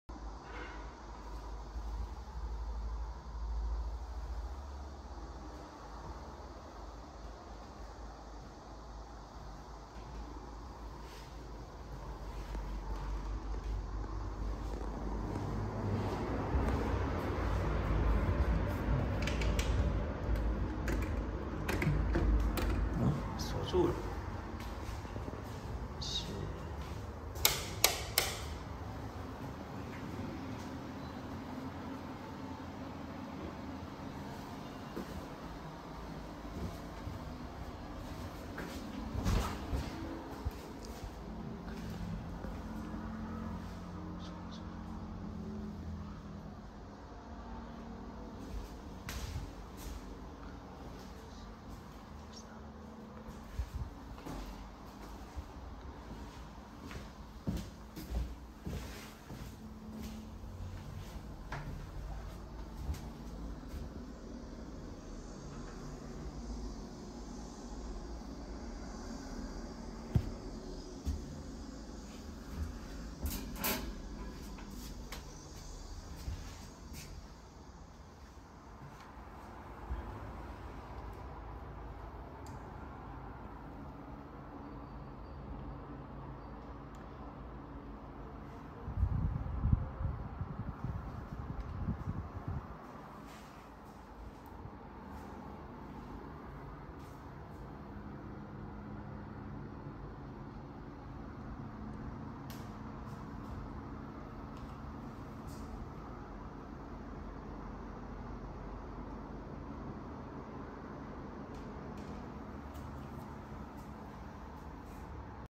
【北京隔離10天】北京防疫隔離公寓環境&防疫物資大解密(COVID-19) in 北京燕保台湖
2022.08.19-【北京隔離10天】北京防疫隔離公寓環境&防疫物資大解密(COVID-19) in 北京燕保台湖